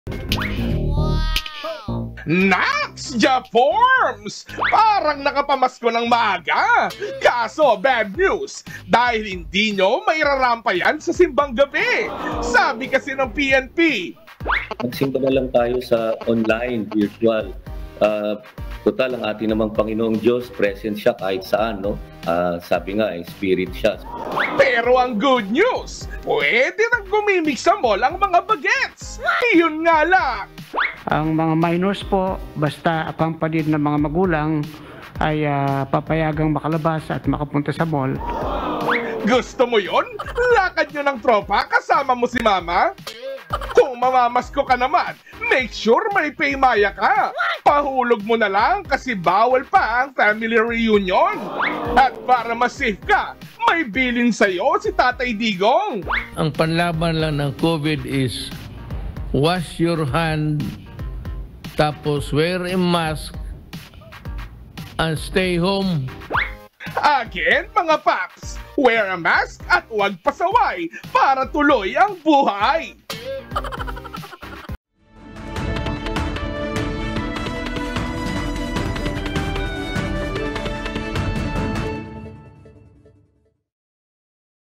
New normal na Pasko
OneBalitaPilipinas | May paalala ang ilang government officials para sa ligtas na pagdiriwang ng Pasko sa gitna ng pandemya.